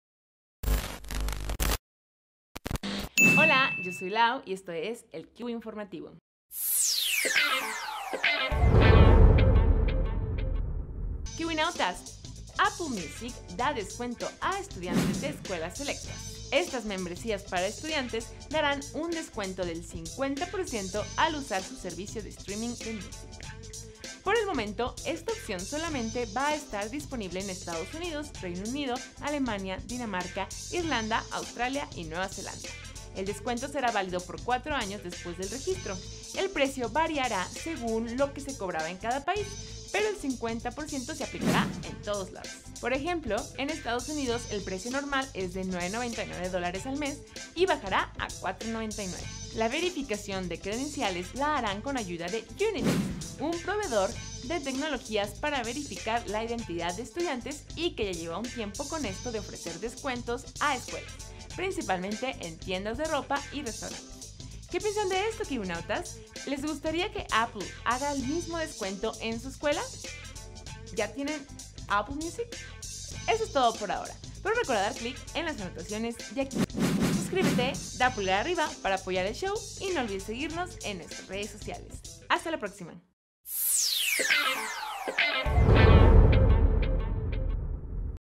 Apple Music da descuento a estudiantes
Los estudiantes podrán gozar de un útil 50% en distintos países

Créditos:

Conducción:
Lau @Laury992

Escrito por:
Oscar Perez @pwrofpwr